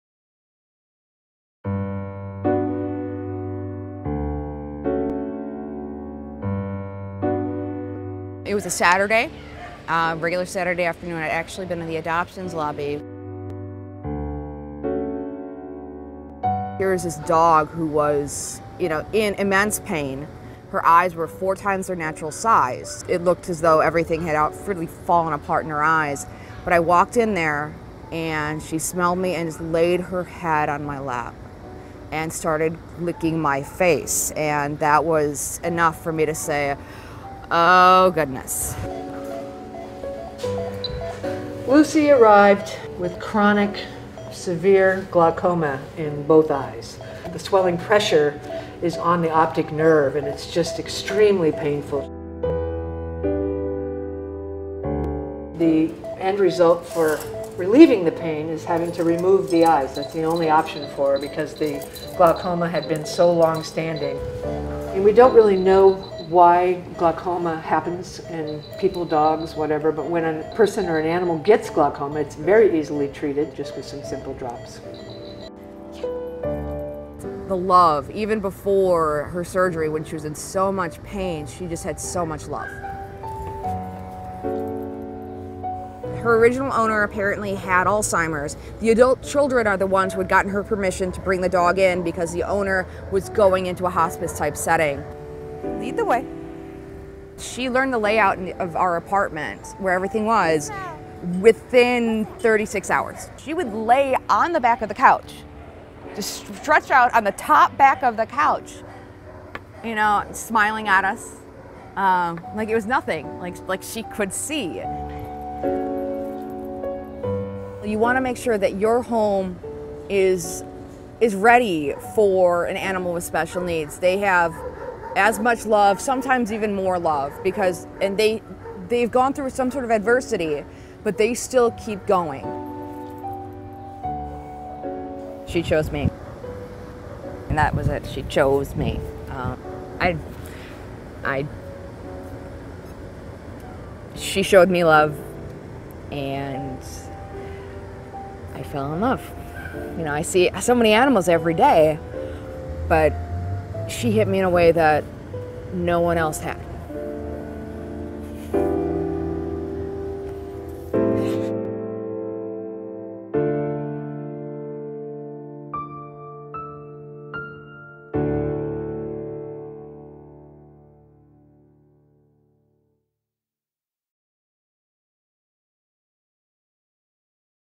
Lucy's Story
Lucy is a blind Bassett Hound who arrived at the Pet Alliance of Greater Orlando in need. To support Lucy and the thousands like her,